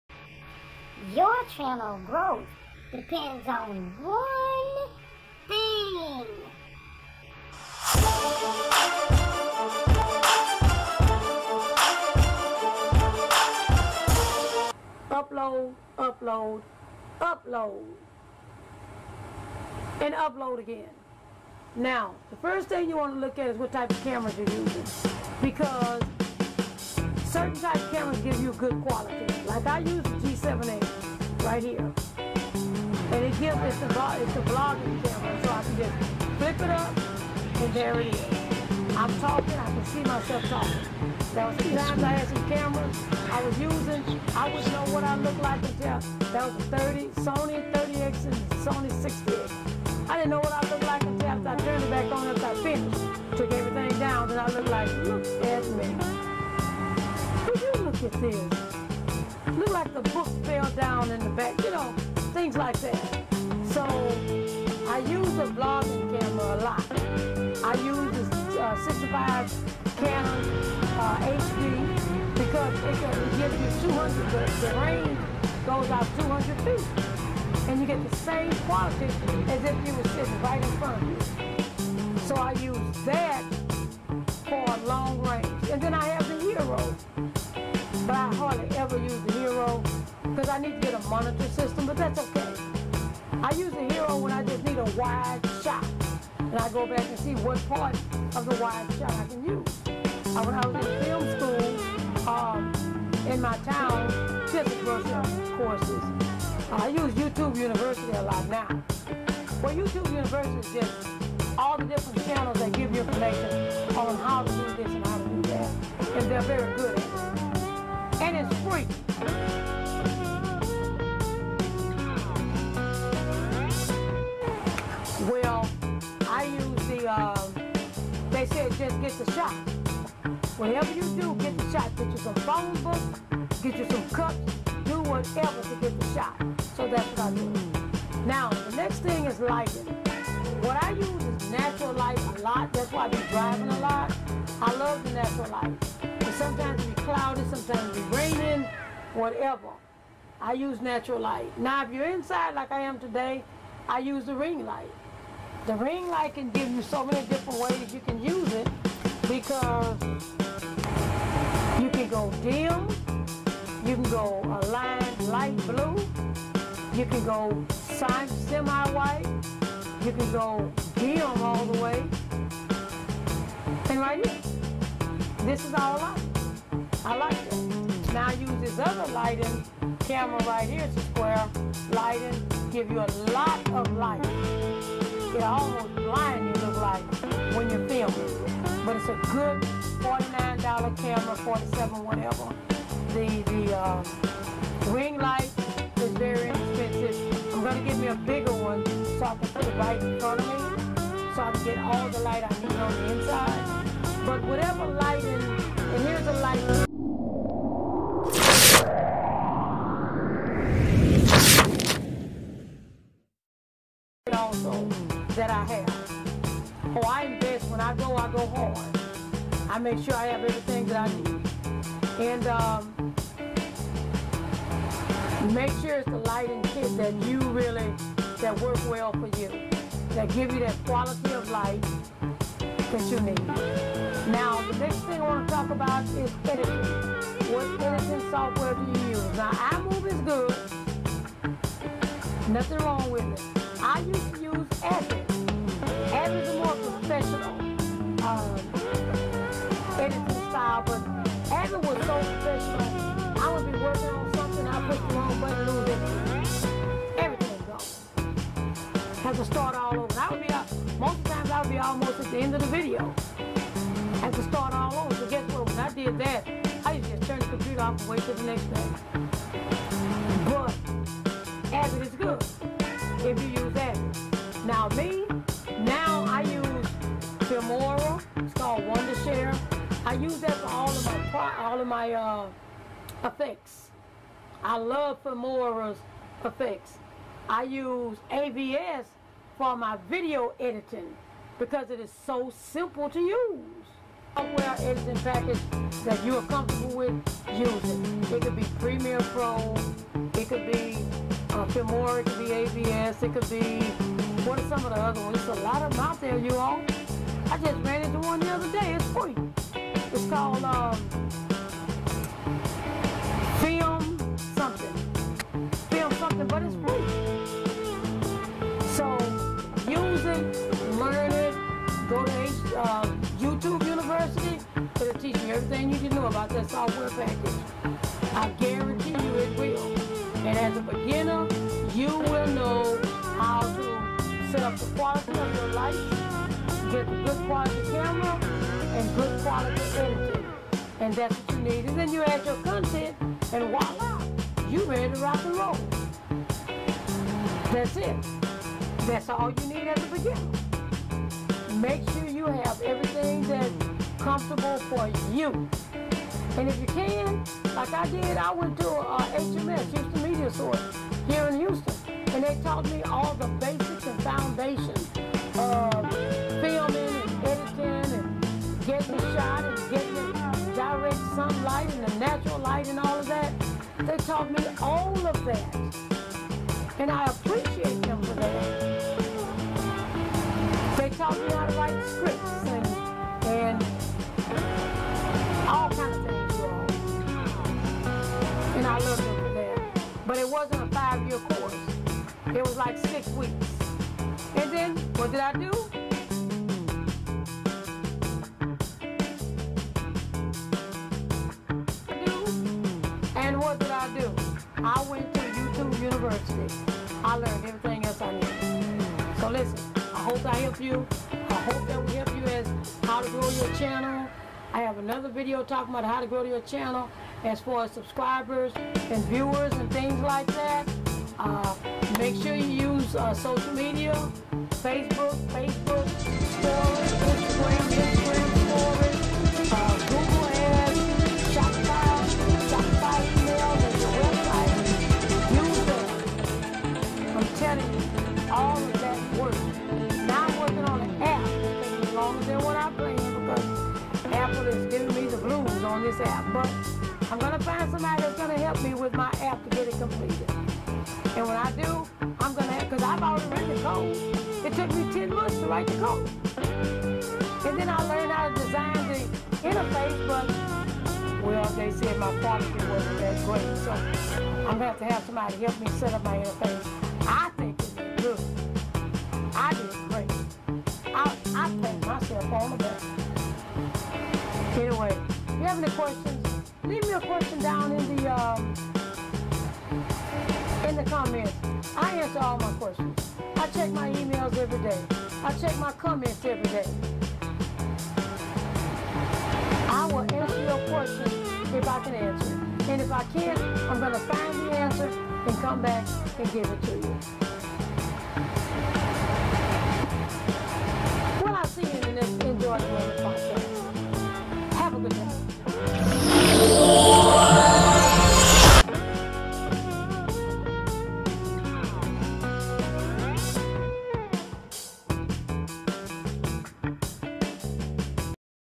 For all Youtube Beginners! Grow your Channel. Braden Brand
For all Youtube Beginners! Grow your Channel. Braden Brand. This video shares the 1 thing that all youtube beginners should know and to help your channel to grow. EnjoytheMoment Video Podcast,

S5/Vlog 380

DISCLAMILER: The Names, Logos and Brand Names in this entire video are non sponsored, and not intended for any promotion.

PIXABAY: Videos/Photos